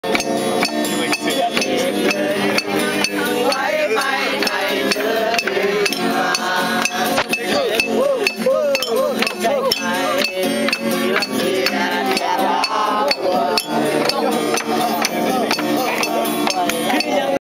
Singing in Pak Beng, Laos
We stopped off in Pak Beng during the 2 day slow boat ride down the Mekong. Late at night (town shuts down at 11:30) we came upon these locals singing and drinking Lao Lao. We chilled for a minute, drank a bit with them, and then went down to the riverside to build a bonfire. Crap quality, but the sound is good for a few seconds.